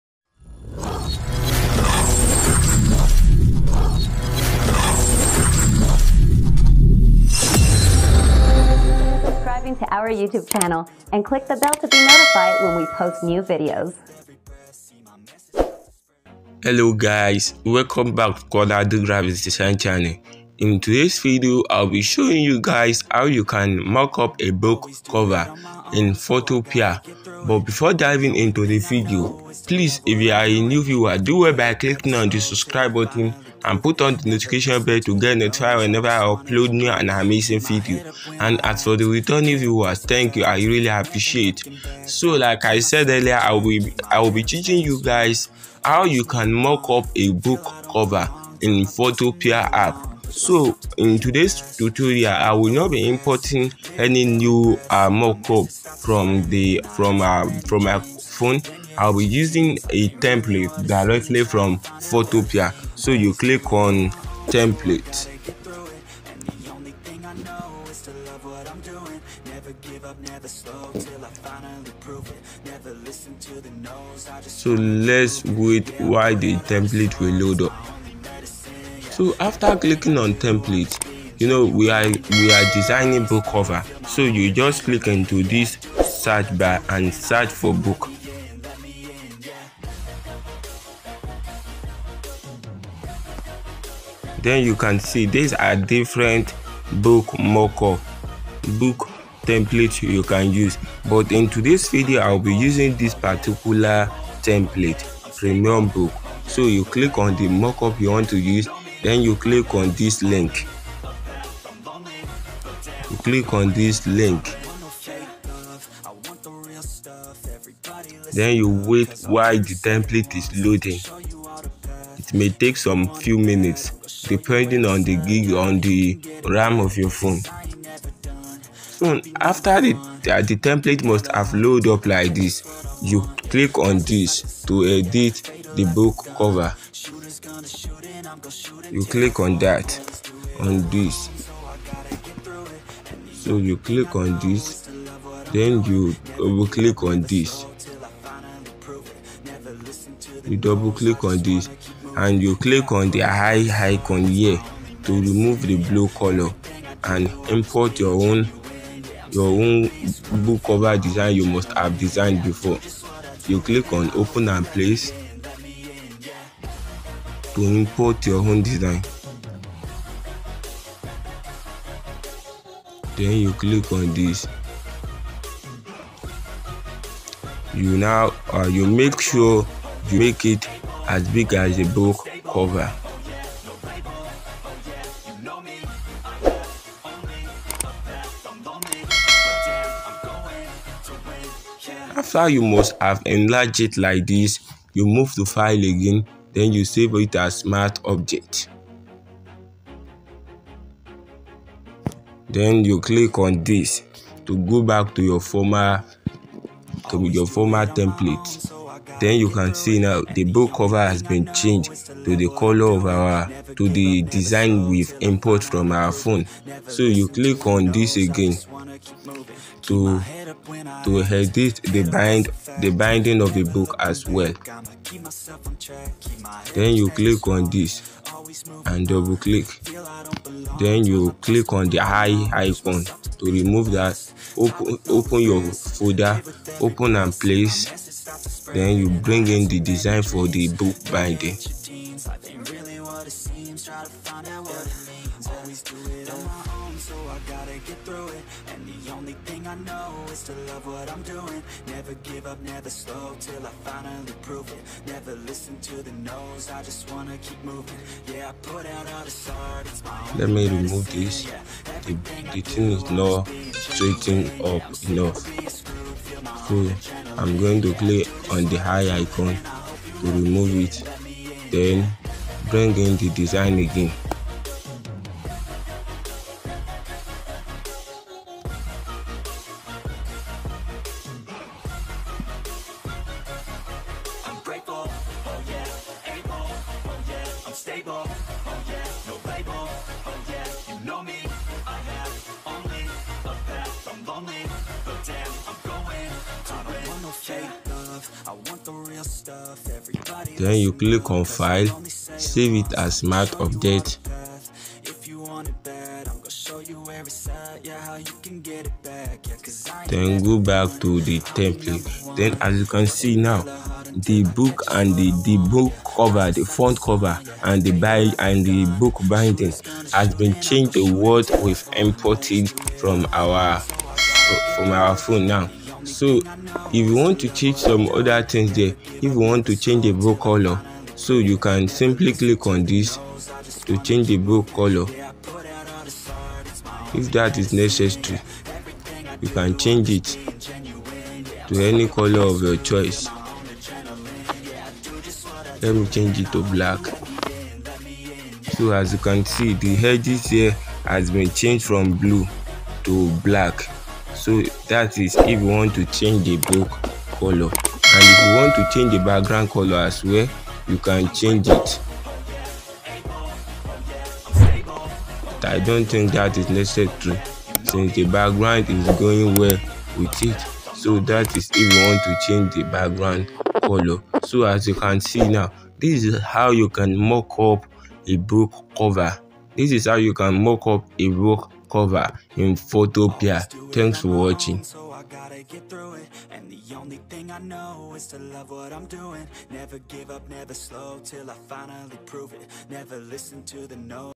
Photopea Tutorial - How to create 3D Book Cover Mockup in Photopea
In this video, you're going to learn how to mockup a book cover using Photopea.

.LET CONNECT.